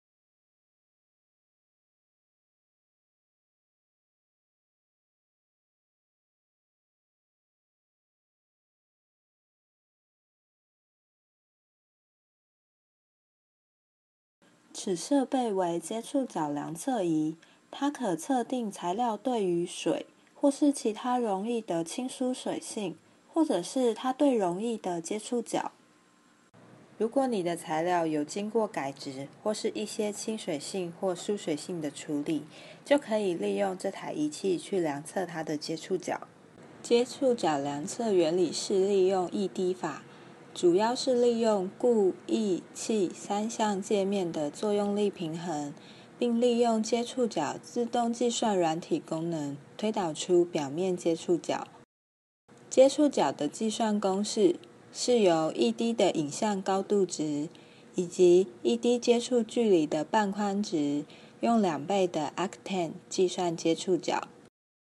接觸角量測儀 (Contact Angle Meter) 概念動畫
國科會科教處奈米國家型科技人才培育計畫經費補助
奈米科技前瞻人才培育整合型計畫執行

國立成功大學微奈米科技研究中心接觸角量測儀器概念動畫